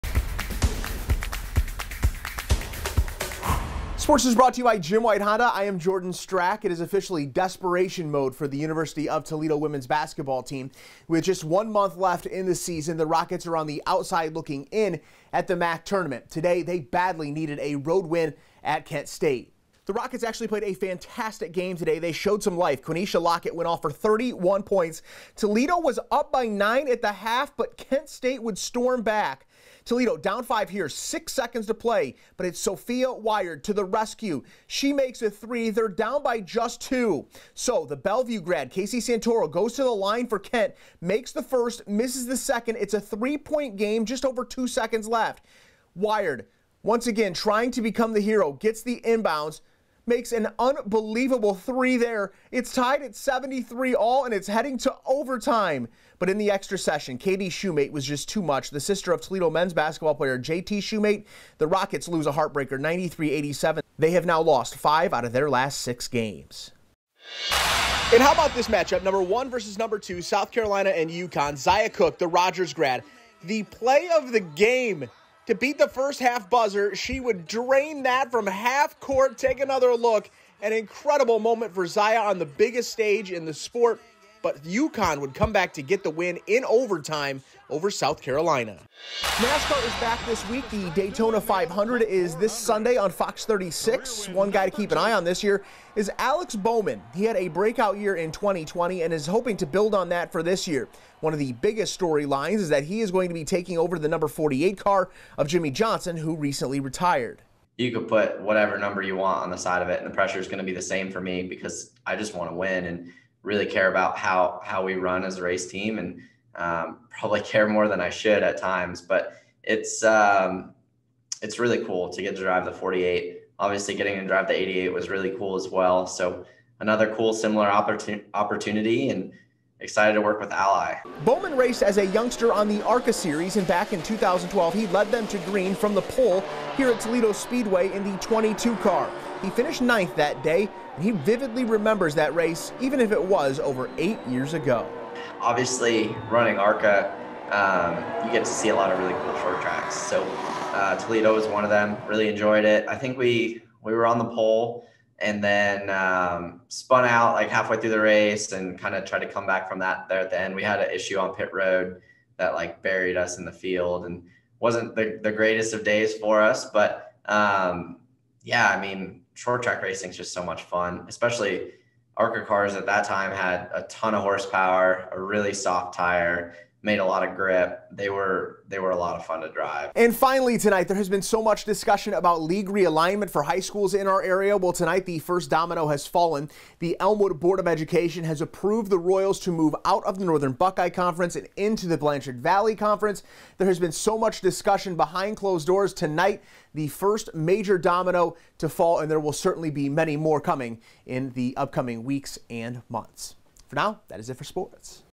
Alex Bowman, taking over the 48 in NASCAR, recalls a day at Toledo Speedway | WTOL Sports - Feb. 8
Sports Director Jordan Strack catches up with NASCAR's Alex Bowman, the new driver of the #48 car. Bowman remembers his 2012 ARCA race - a rough day at Toledo Speedway.

Plus: Zia Cooke sinks an incredible half-court halftime buzzer beater and the Elmwood Royals the first domino to fall for conference realignments.